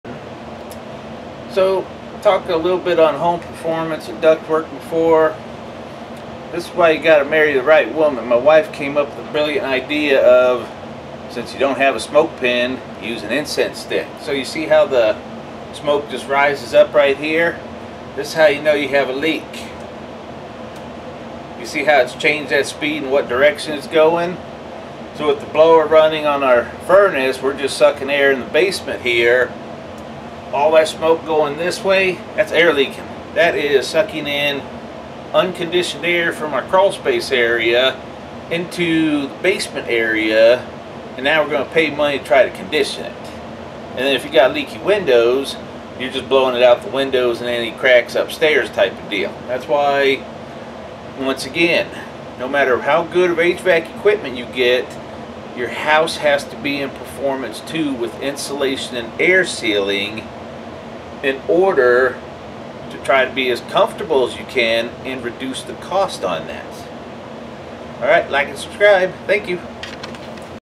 Cheap meathod to test for air leaks on a house with your furnace blower
Do you feel the draftiness of your house? Do you have high utility bills? I demonstrate the cheap was with an incense with my furnace fan (blower) running. If the smoke moves, you have an air leak.